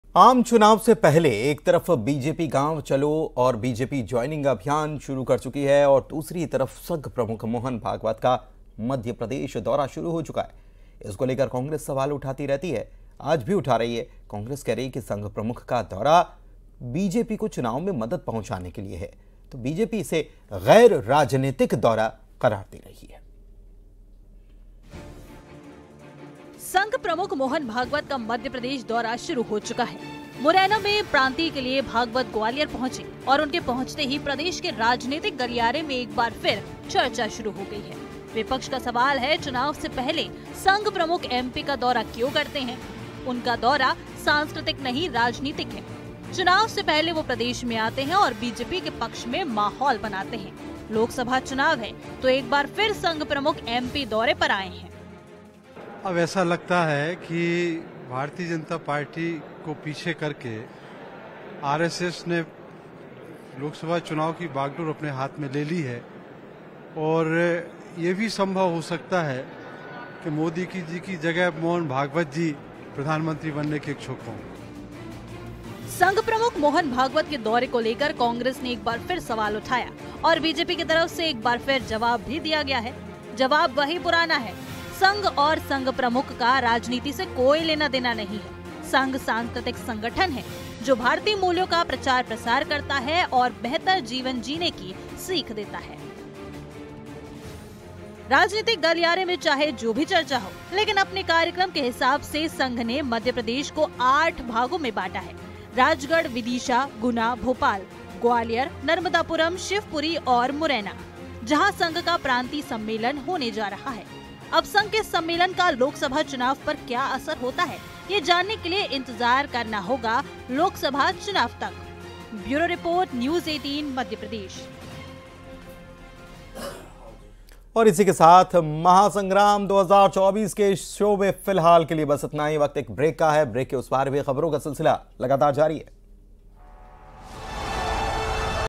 Lok Sabha Election 2024 : Mohan Bhagwat का MP दौरा, Congress ने उठाए सवाल | BJP | Latest News
Mohan Bhagwat का MP दौरा, Congress ने उठाए सवाल, तो वहीं मोहन भागवत ने कहा यह सास्कृतिक दौरा है.